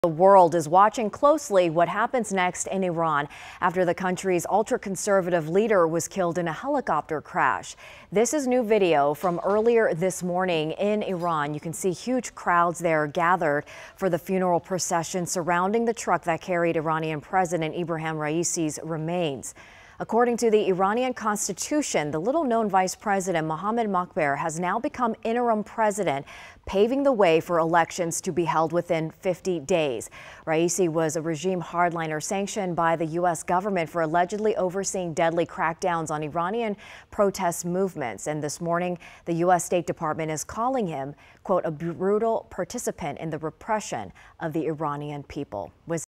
World watching after Iran's president's death
Elections will be held within 50 days.